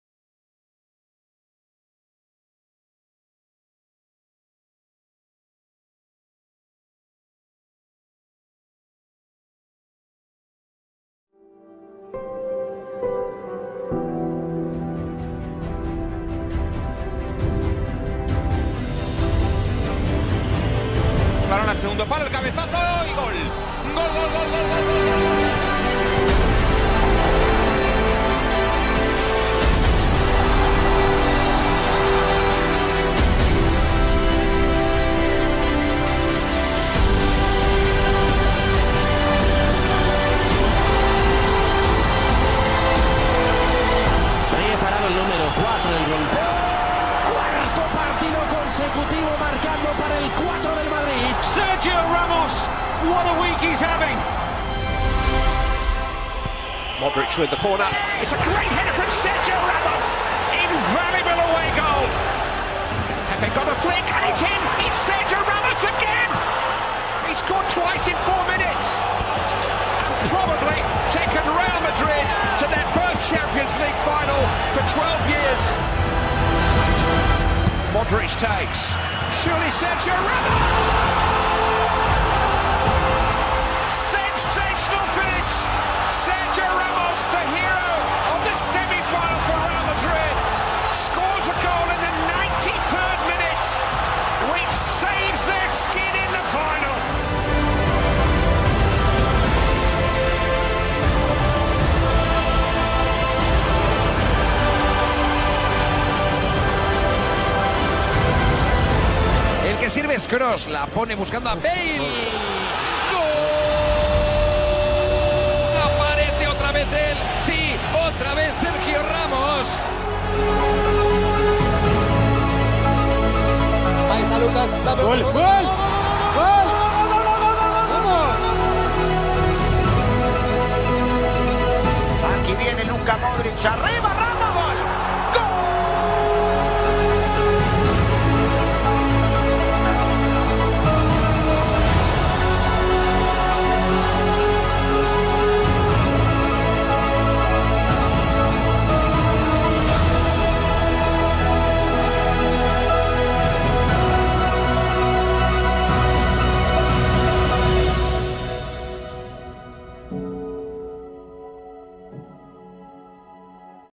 Despedida del capitán Ramos, Real Madrid
Mira como fue la despedida del Capitan Sergio Ramos